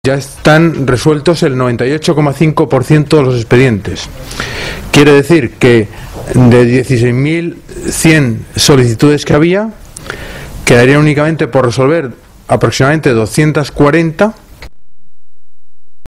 La Comisión Mixta autoriza ayudas para viviendas dañadas por los terremotos
La Comisión Mixta, creada por el Real Decreto-Ley 6/2011 sobre adopción de medidas para reparar los daños causados en Lorca por los movimientos sísmicos de mayo de 2011, aprobó un total de 71 solicitudes de ayudas a la vivienda, que suman subvenciones por valor de 982.732 euros. En esta ocasión se ha aprobado una sexta partida de ayudas a la reposición de enseres que consta de seis expedientes (8.714 euros) que, sumados a los aprobados en las reuniones anteriores de la Comisión Mixta por este mismo concepto, hacen un total de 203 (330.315 euros). El consejero de Obras Públicas y Ordenación del Territorio, Antonio Sevilla, indicó que los expedientes estudiados en las reuniones resolutivas de vivienda de la Comisión Mixta suman 15.875, que supone el 98,5 por ciento del total, cuyo montante económico aprobado hasta la fecha asciende a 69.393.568 euros.